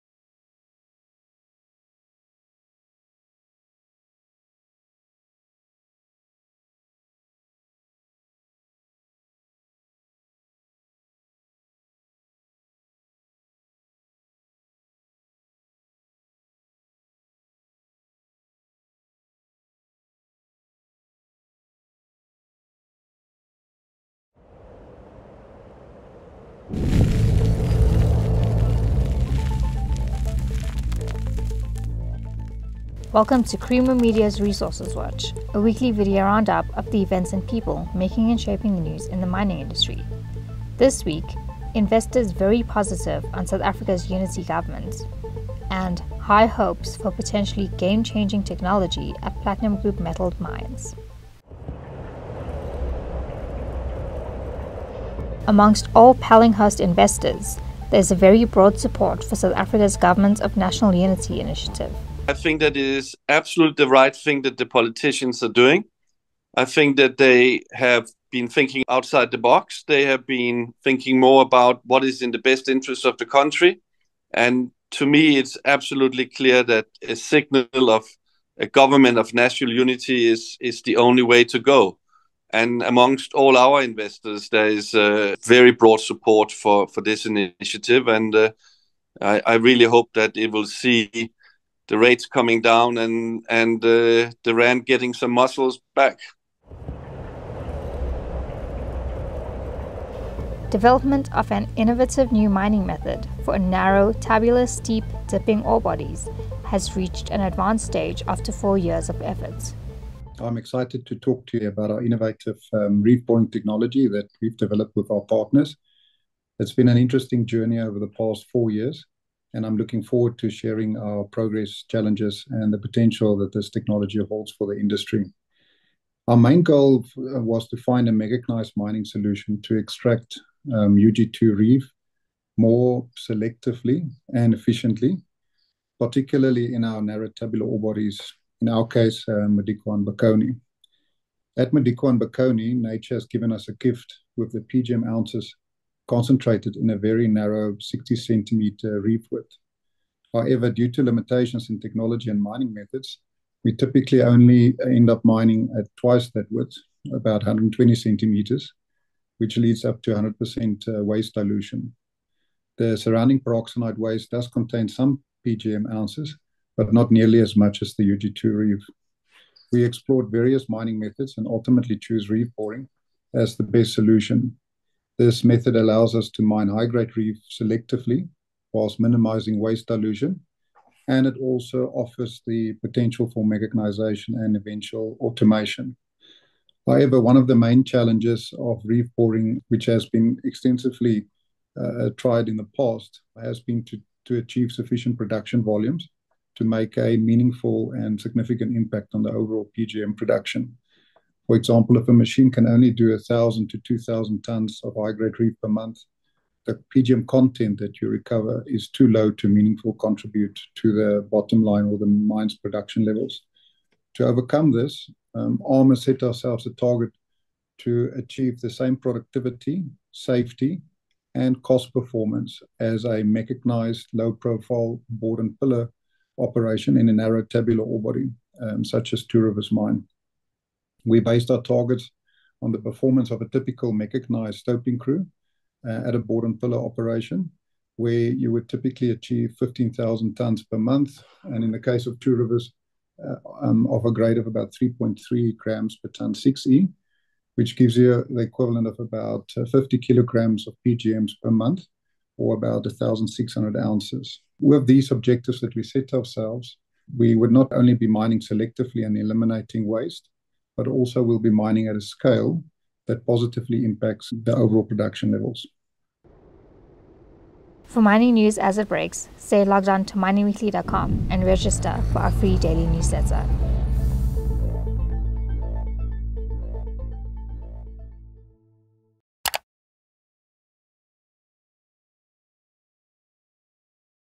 Resources Watch
This week: Investors very positive on South Africa's unity govt, says Pallinghurst's Arne Frandsen and, High hopes for potentially game-changing technology at platinum group metal mines